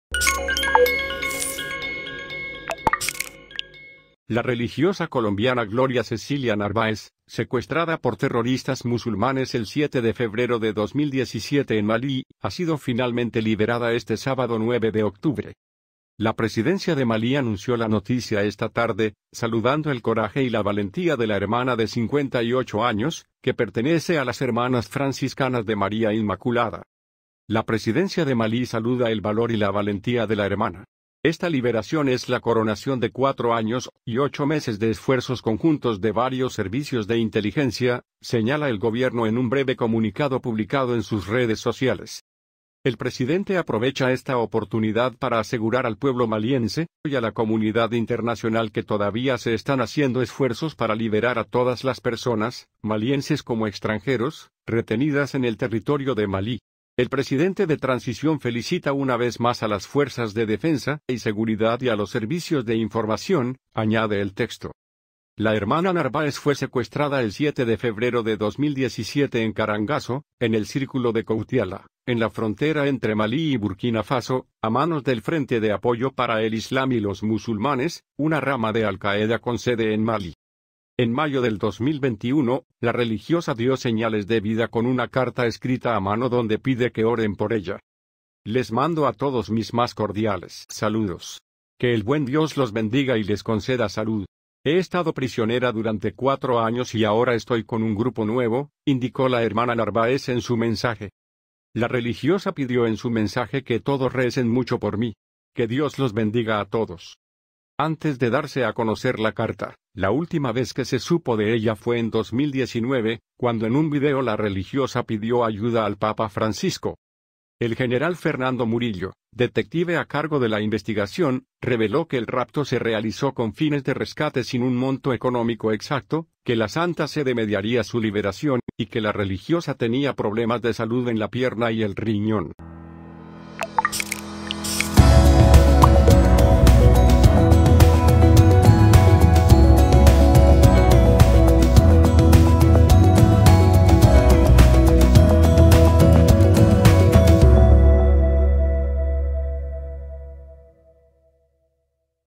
🔴ÚLTIMA HORA: Liberan a religiosa colombiana GLORIA CECILIA NARVÁEZ tras más de 4 años de secuestro
La religiosa colombiana Gloria Cecilia Narvaez Argori, secuestrada por terroristas musulmanes el 7 de febrero de 2017 en Malí, ha sido finalmente liberada este sábado 9 de octubre.